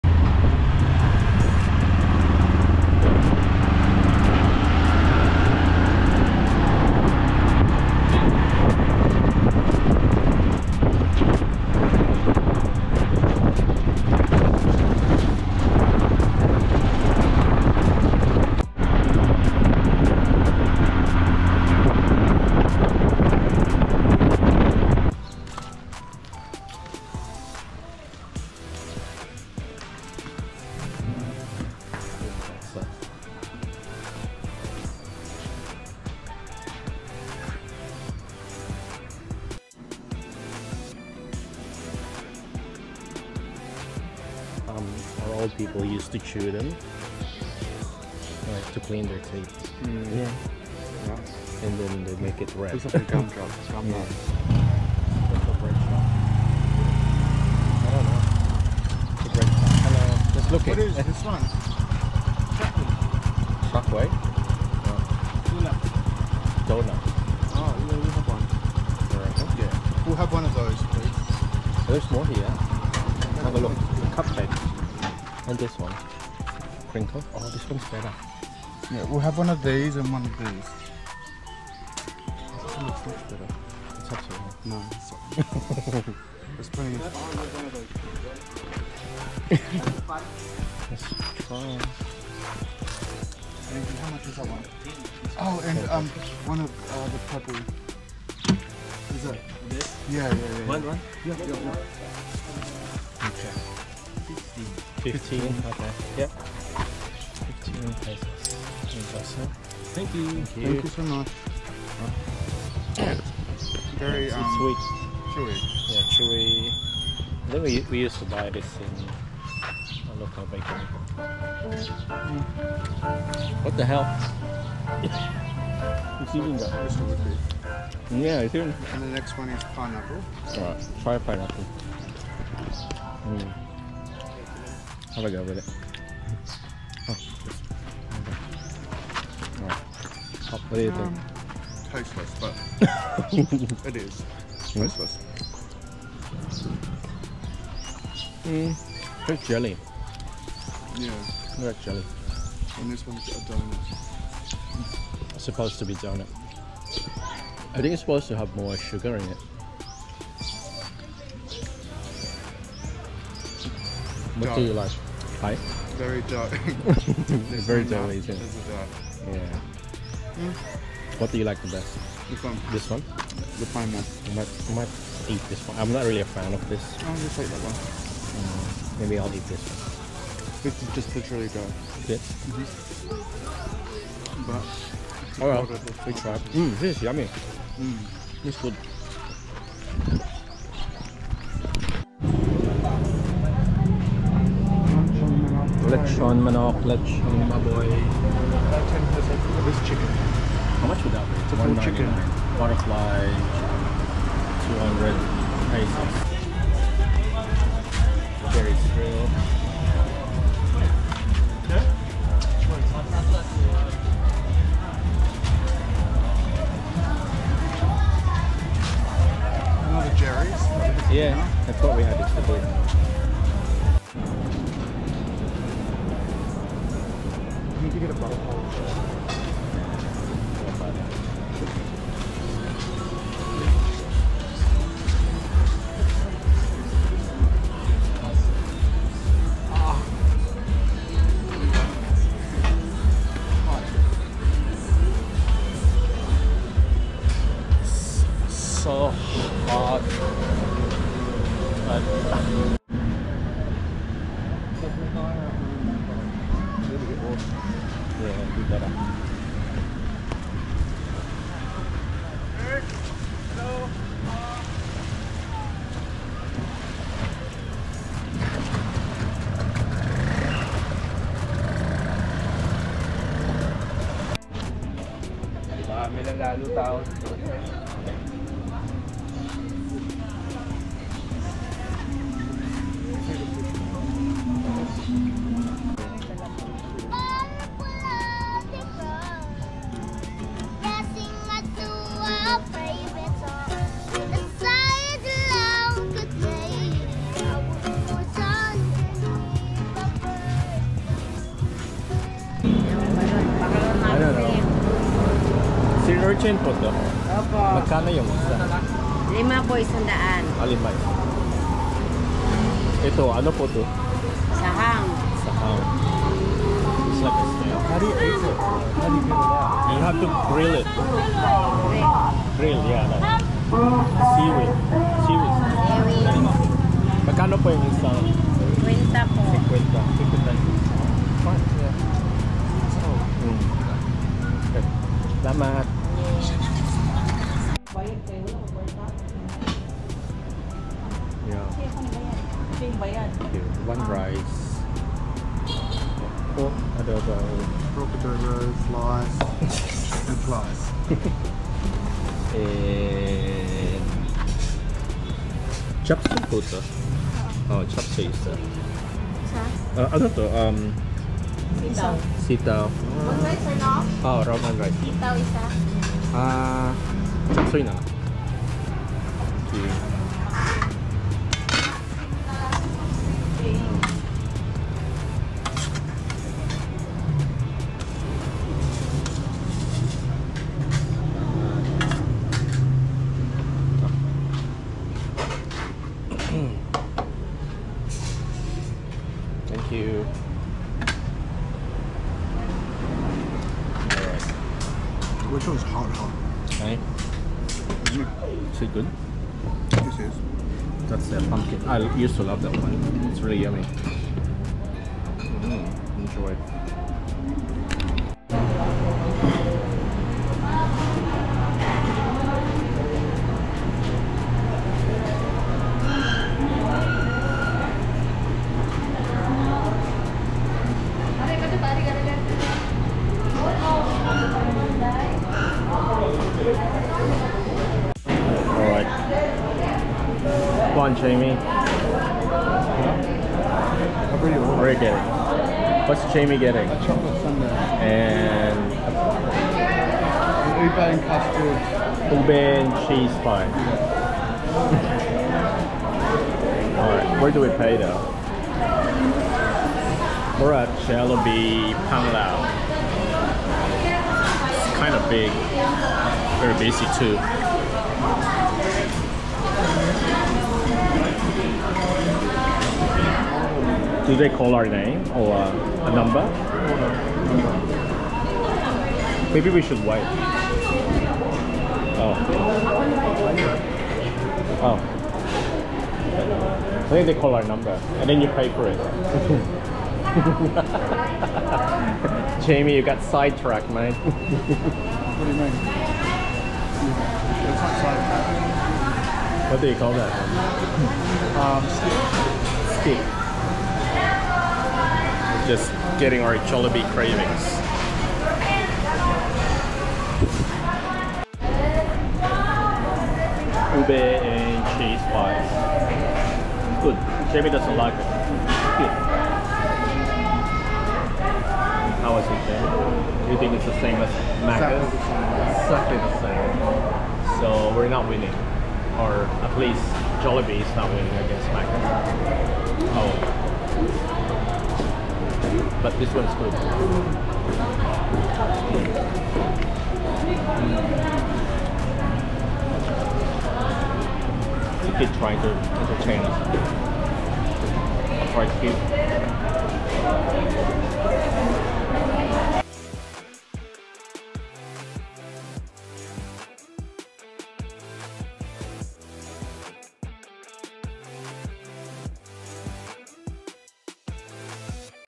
Panglao and Alona beach
We attempt to explore the local beach vibe at Alona beach where we booked at Tamarind hotel. Alone beach is definitely busy touristy. However, we enjoyed the local beach where you could relax and find some cheap and interesting treats!